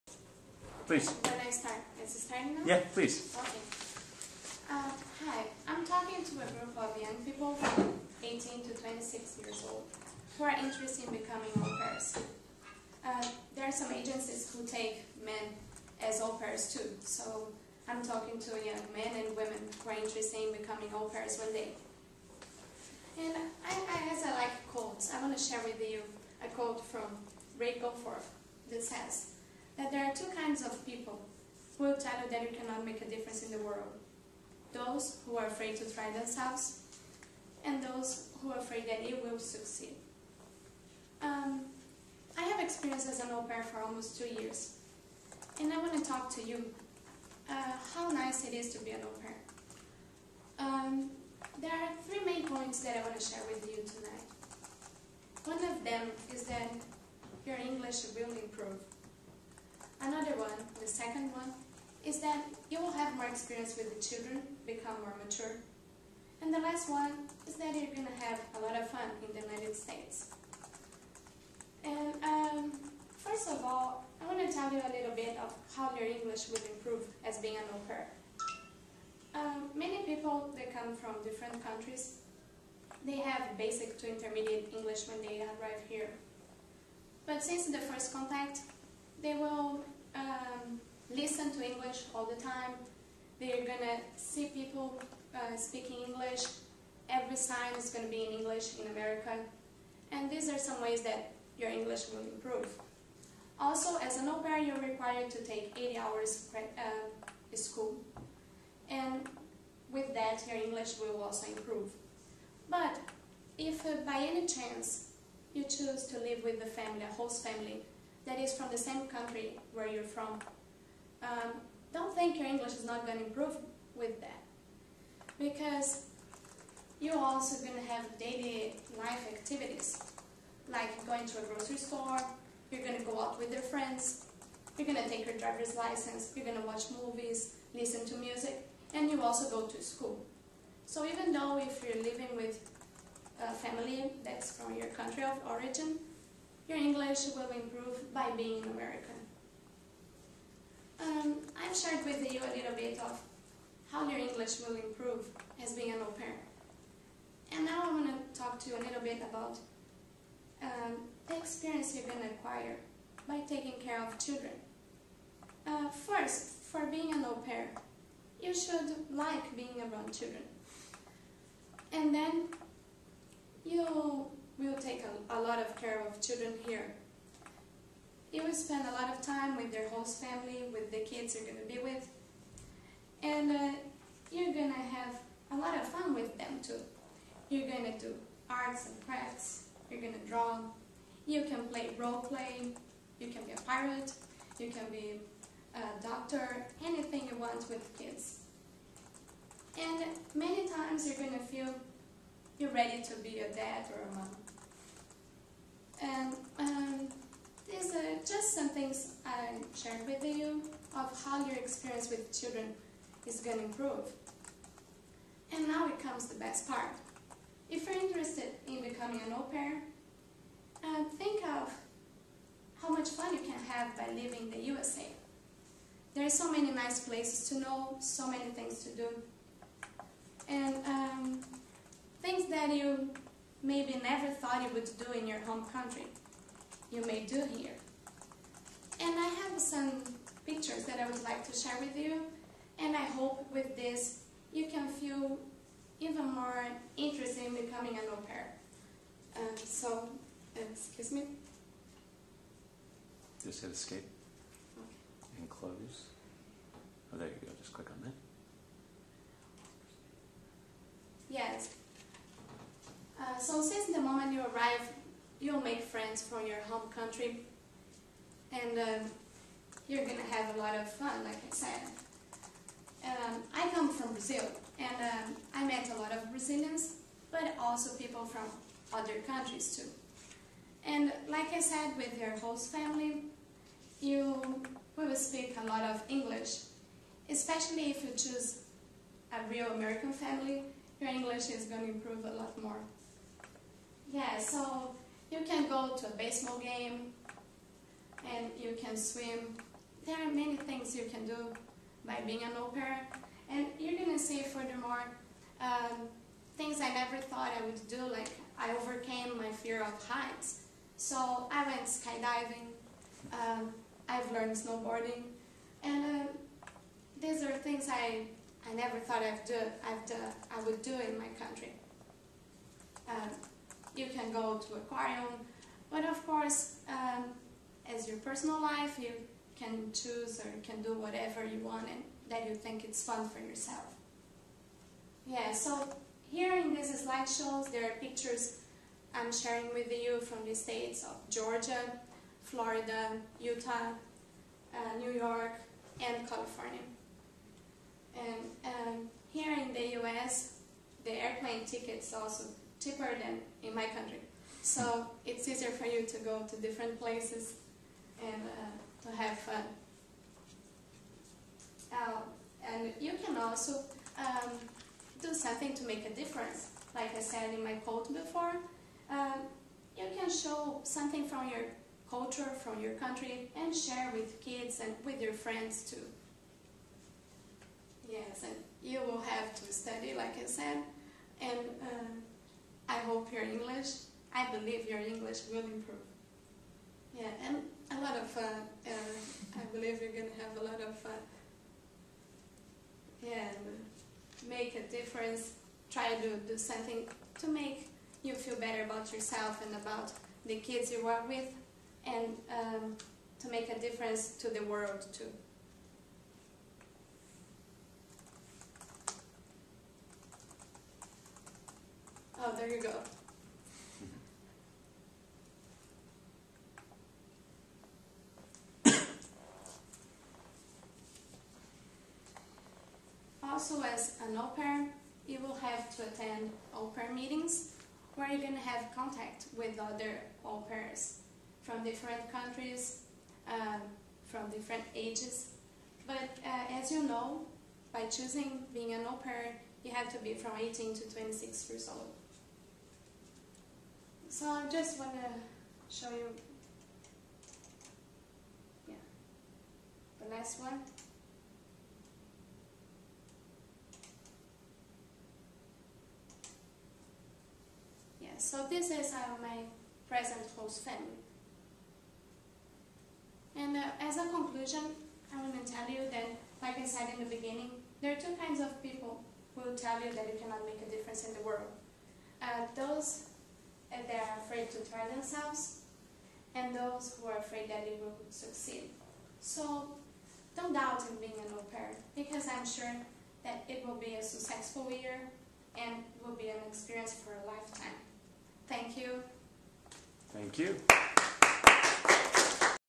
Be an Au Pair!! - Presentation at Stanford
Speech at Public Speaking Class in Stanford University, CA on Dec 01, 2009.